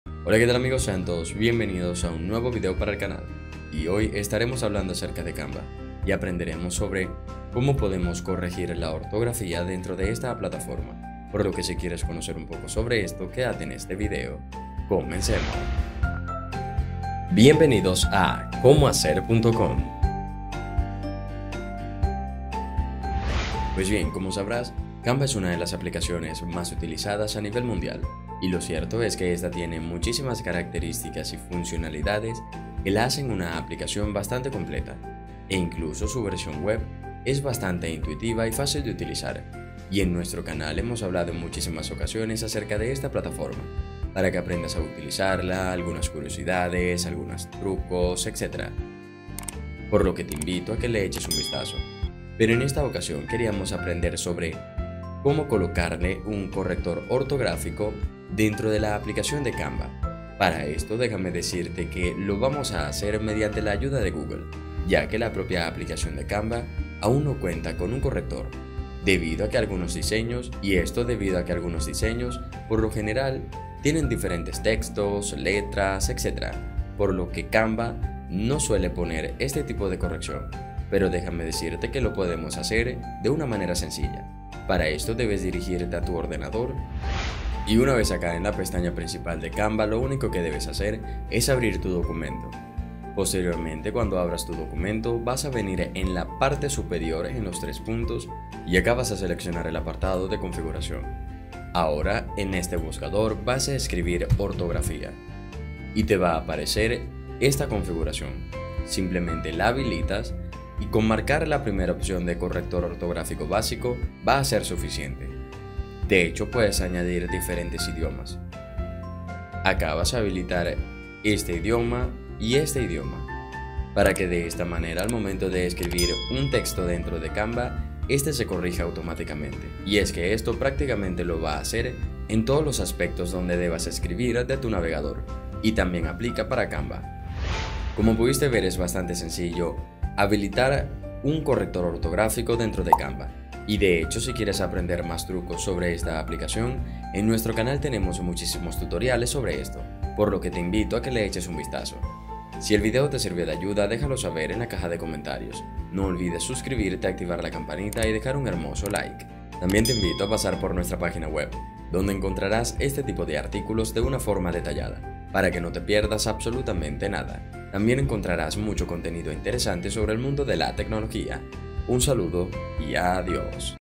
Cómo CORREGIR LA ORTOGRAFÍA DE tus Diseños en Canva - ✔️​ Activa esta Herramienta ✔️​
¿Realizas muchos diseños en Canva con bloques de texto? En ese caso, de seguro te ha pasado que al terminar te das cuenta de palabras mal escritas. No te preocupes, puedes evitar que eso suceda, y en este video te mostramos cómo hacerlo. Aprende Cómo CORREGIR LA ORTOGRAFÍA DE tus Diseños en Canva. Conoce la forma de activar el corrector ortográfico en Cava de forma sencilla y corregir tus diseñaos automáticamente.